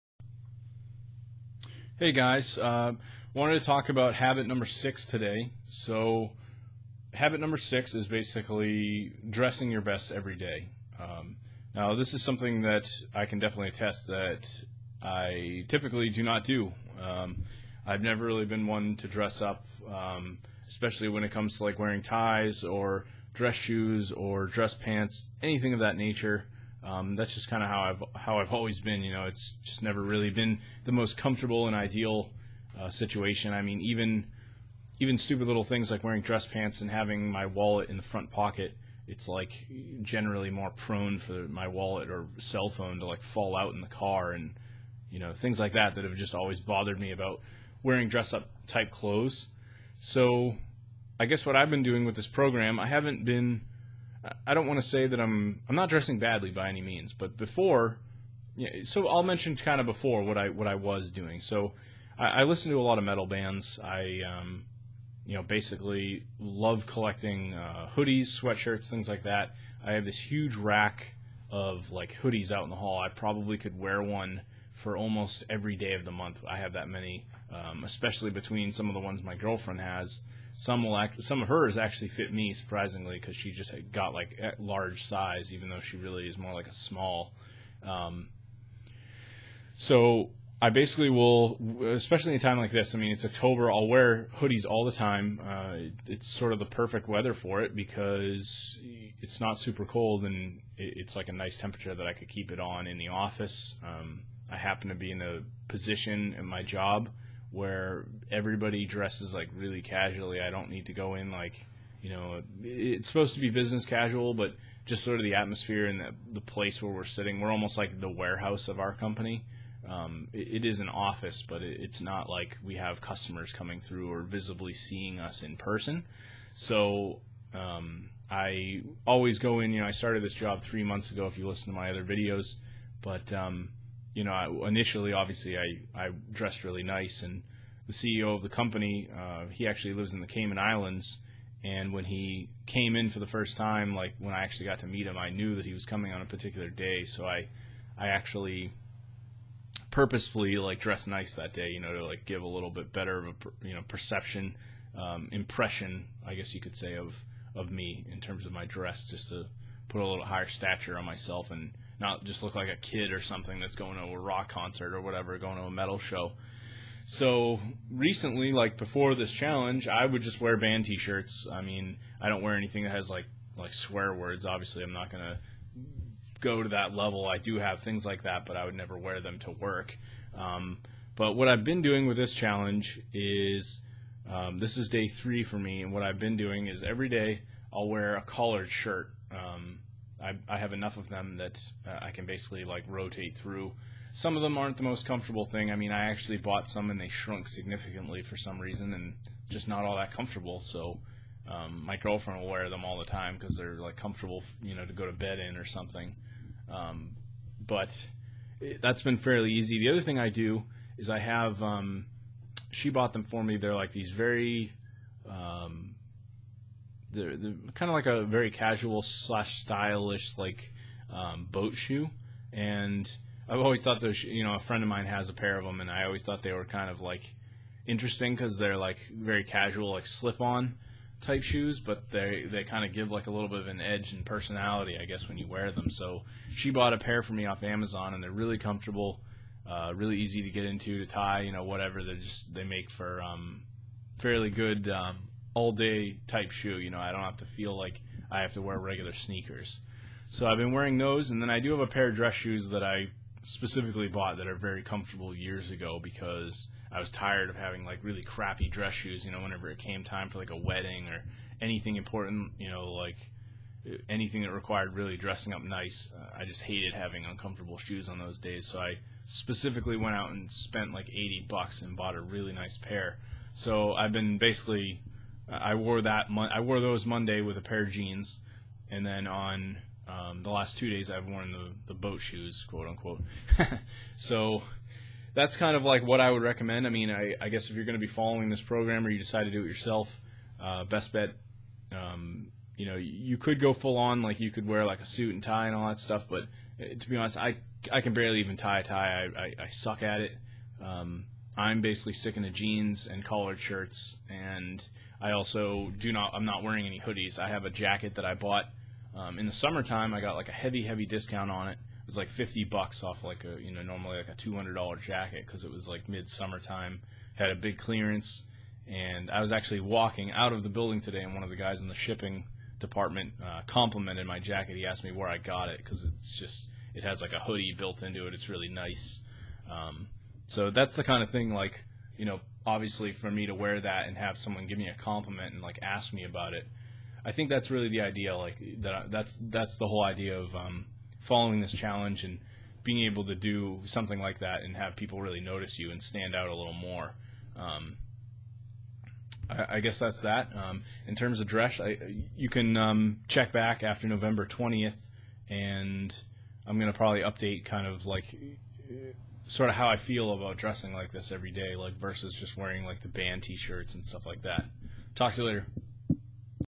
Habit #6 - Dress Your Best Everyday - 30 Days Of Discipline Journey
I talk about the 6th habit in 30 days of discipline, dressing your best every day.